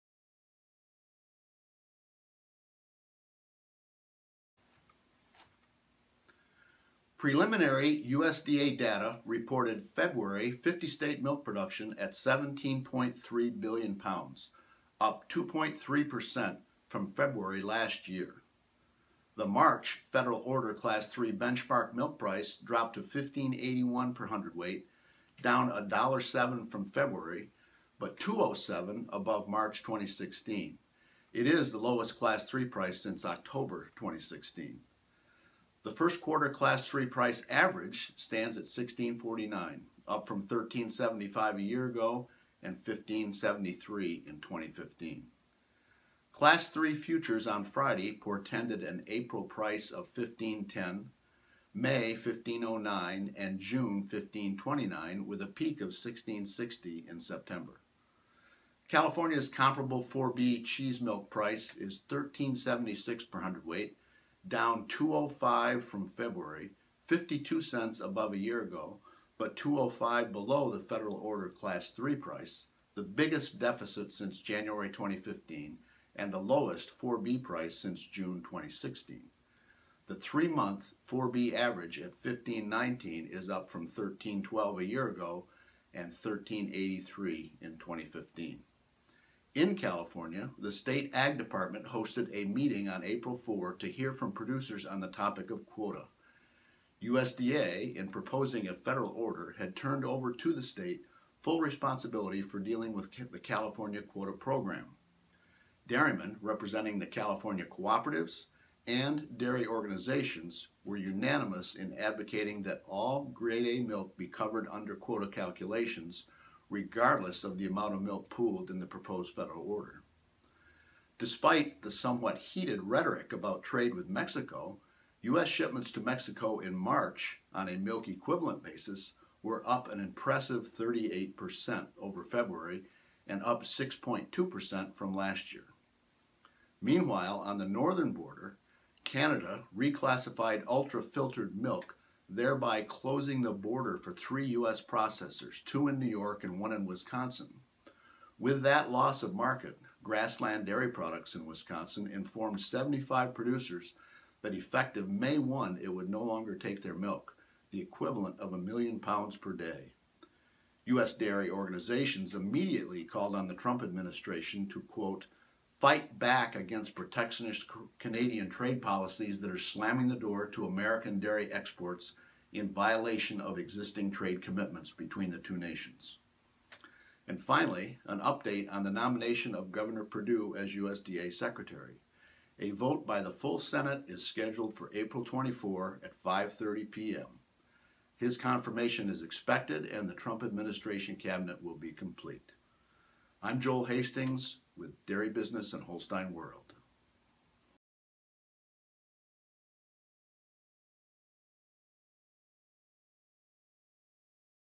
Dairy Week News in Review by Dairy Business April 7 2017
The dairy business week in review by Joel Hastings of DairyBusiness & HolsteinWorld.